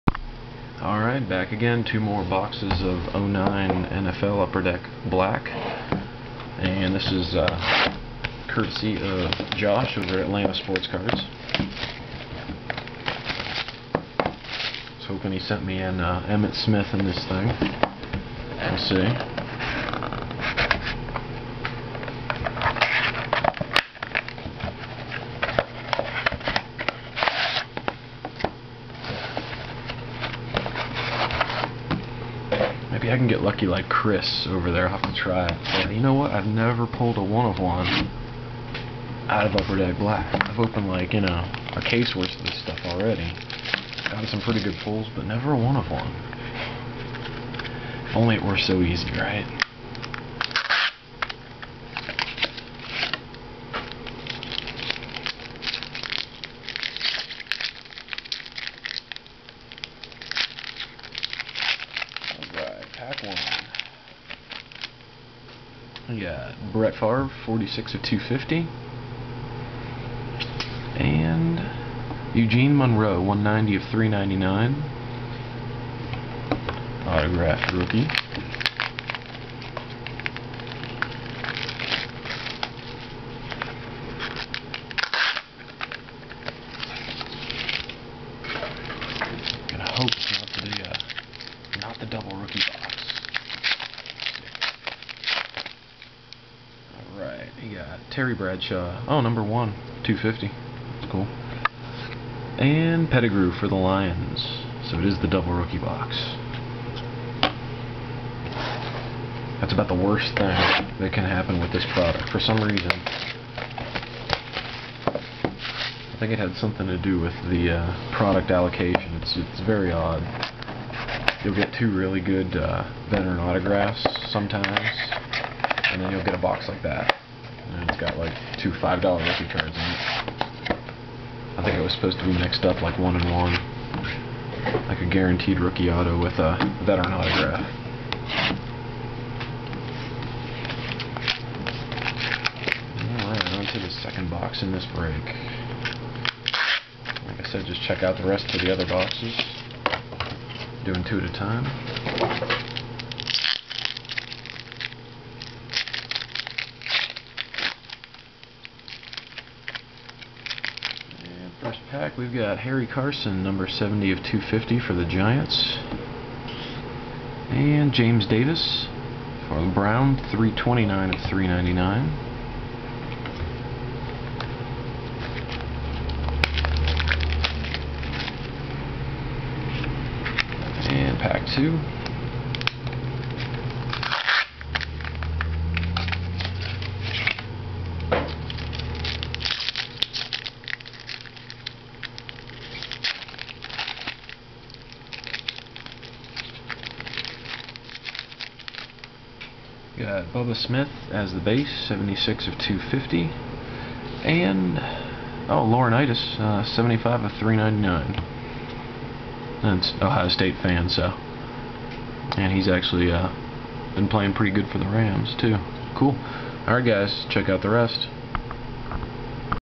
2009 UD Black NFL CASE BREAK 3 of 5
2 box break of 2009 UD NFL Black.  Thanks to Josh at Atlanta Sportscards!